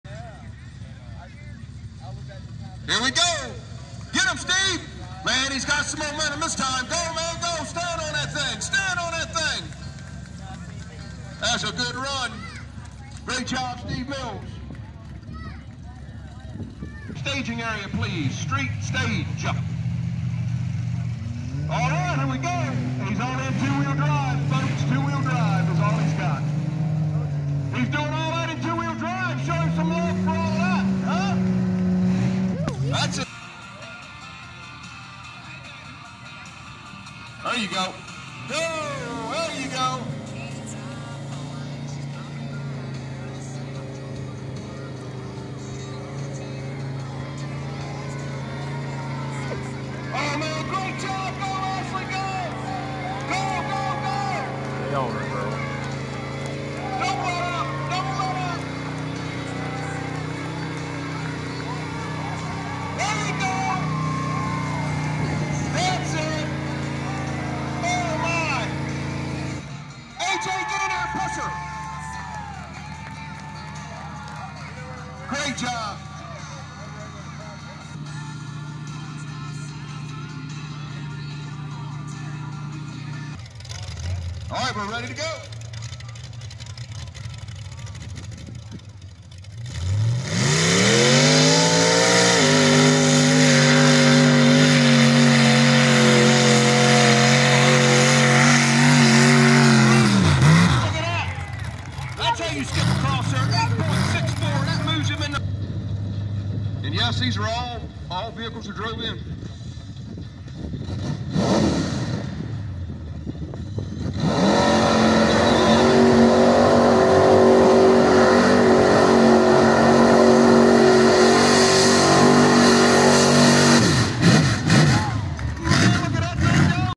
Best of street class Pocomoke City fair grounds July 1 2023 mud bog racing
Out of order and 2nd run for a couple. best runs made by street class trucks. everyone put on a awesome show and all these trucks pulled hard in that mud pit. great day mud racing.